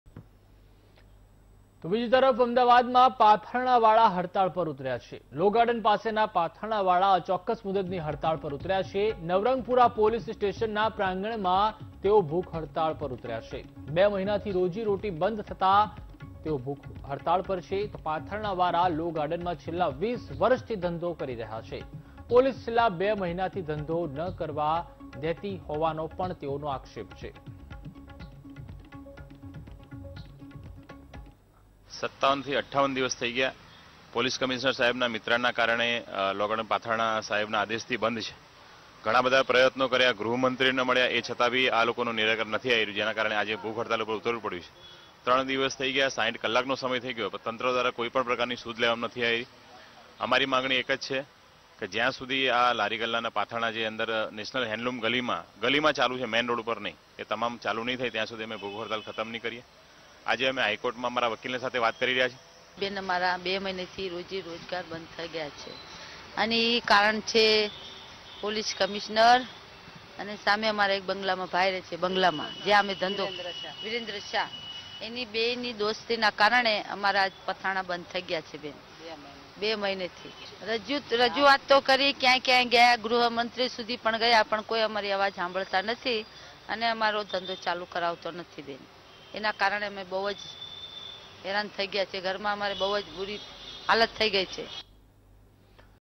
Ahmedabad માં પાથરણાવાળા હડતાળ પર ઉતર્યા, 2 મહિનાથી રોજી-રોટી બંધ થતાં ઉતર્યા ભૂખ હડતાળ પર
Ahmedabad માં પાથરણાવાળા હડતાળ પર ઉતર્યા, 2 મહિનાથી રોજી-રોટી બંધ થતાં ઉતર્યા ભૂખ હડતાળ પર | VTV GUJARATI